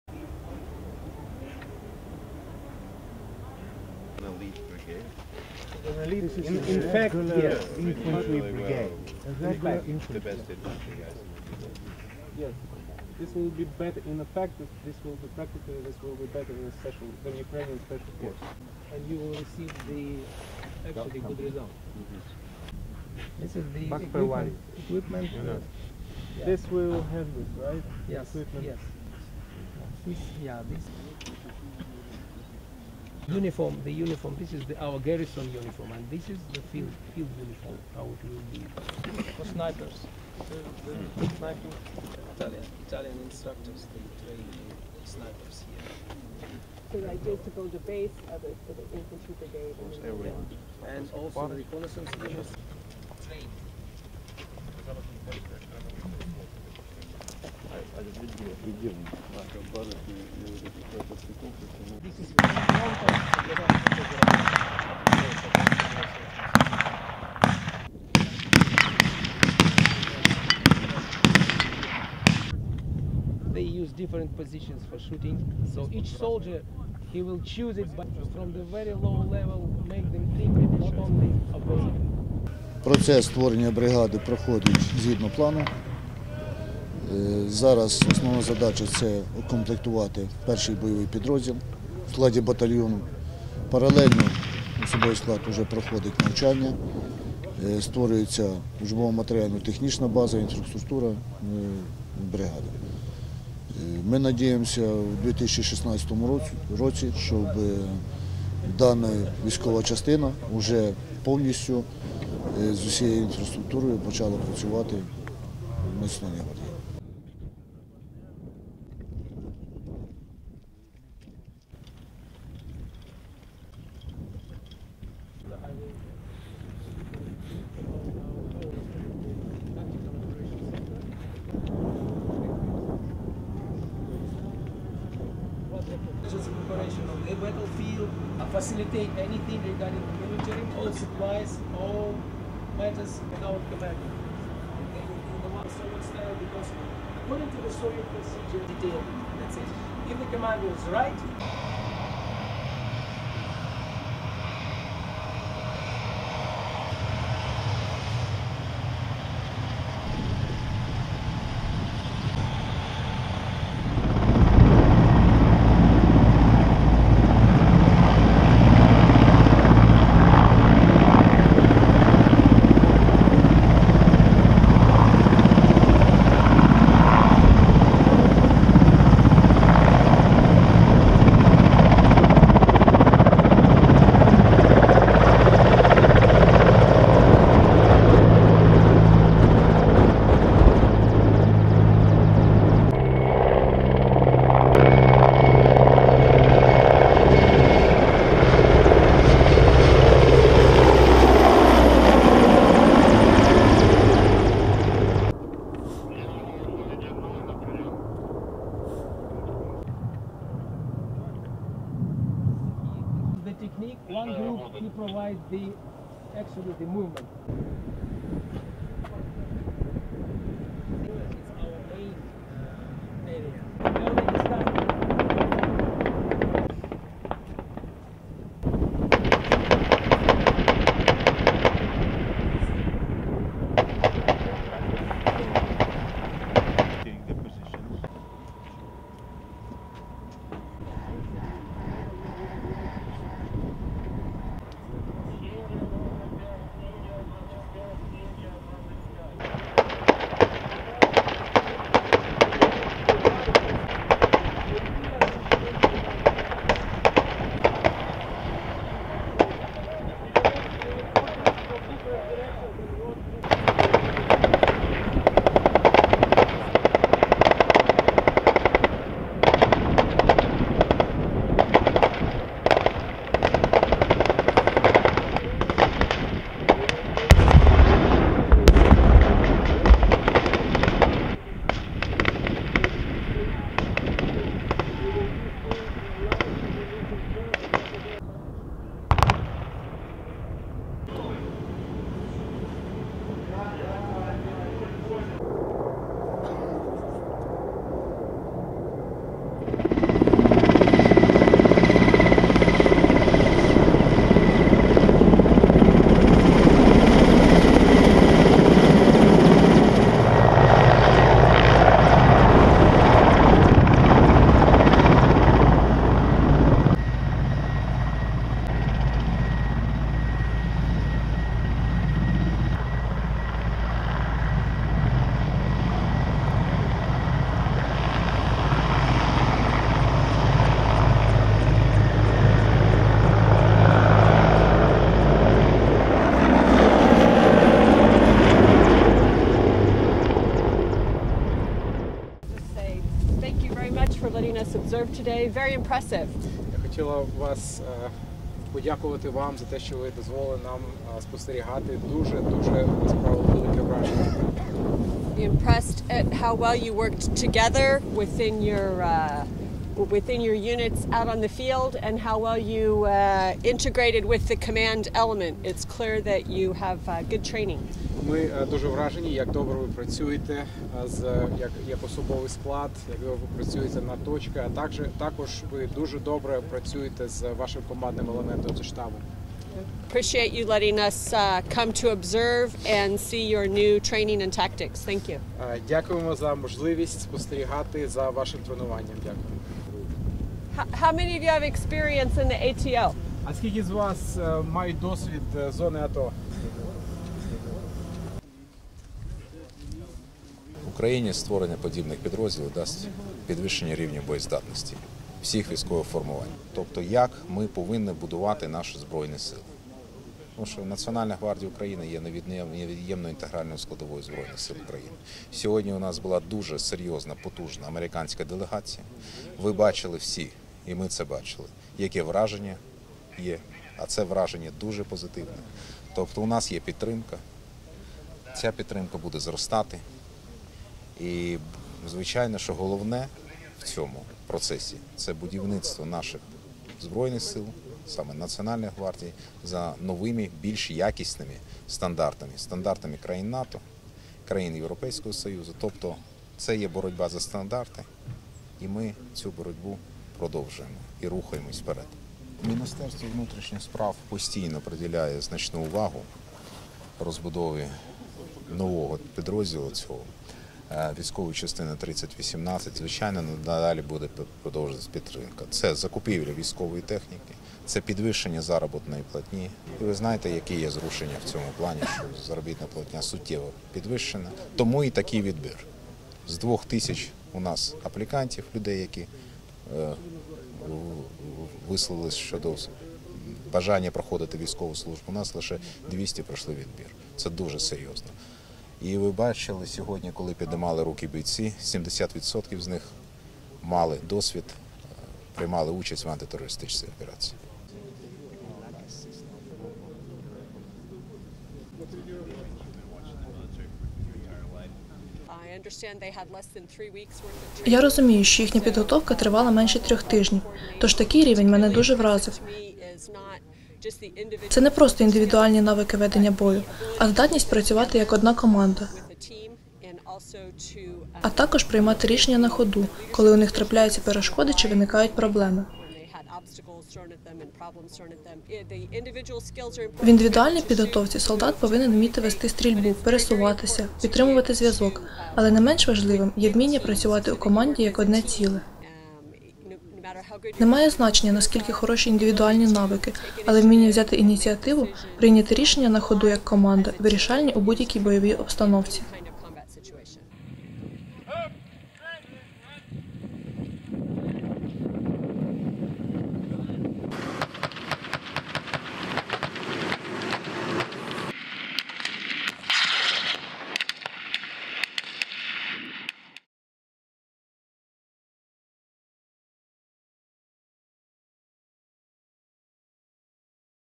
Нацгвардійці продемонстрували бойову підготовку
21 грудня на полігоні Нацгвардії у Нових Петрівцях було проведено показове заняття за участю військових аташе посольства Сполучених Штатів Америки в Україні.

Військовий аташе з питань армії та оборони полковник Керол Нортрап, керівник військово-політичного напрямку Даглас Хойт, політичний аналітик Олексій Павлов та командування НГУ оглянули бойове злагодження і тактичні навички новоствореної бригади легкої піхоти Нацгвардії.